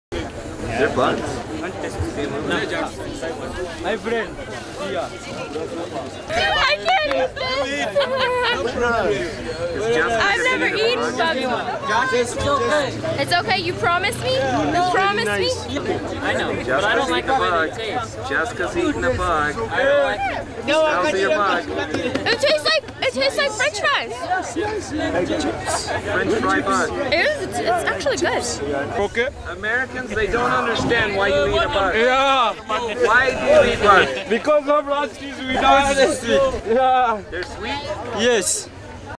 My daughter ate a bug in Africa
Bugs are sold on the streets of Africa. When they see an American, they hurry to offer you one for free because they know we are squeamish about bugs. My daughter was no exception.